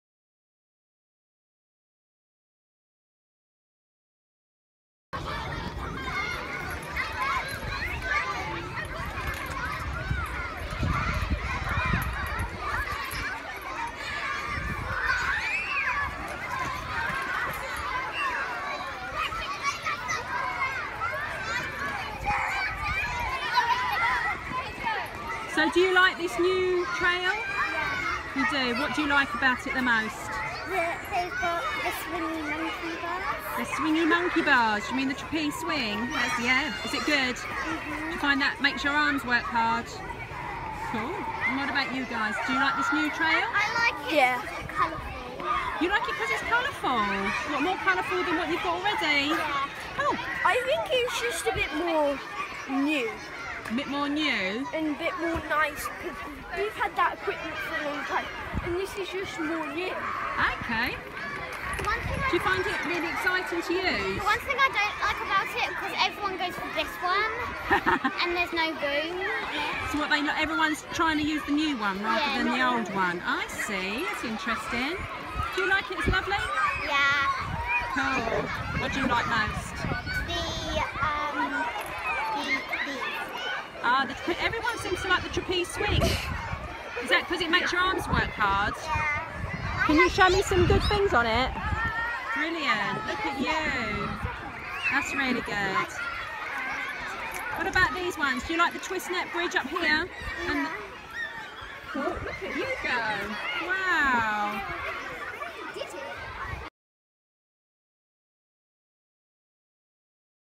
Homer First School's New Trim Trail
We worked with Homer First to provide them with a fantastic new trim trail that has been hand picked to work alongside their already existing combination trail. Choosing resources that would engage and test the children with new and exciting trail items to develop from.